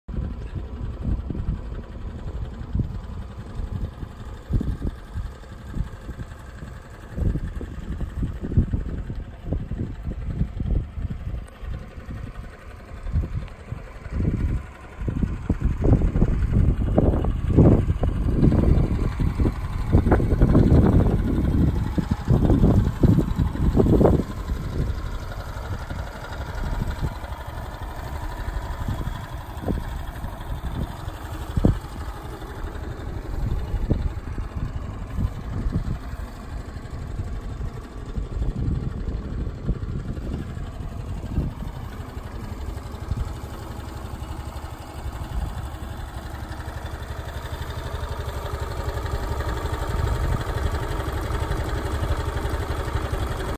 MyCar.mu - Suzuki Burgman Street 125
Light GT

La gamme des scooters Suzuki vient de s’enrichir avec l’arrivée du Burgman Street 125 qui va faire la passerelle entre les gros scooters GT et les petits urbains.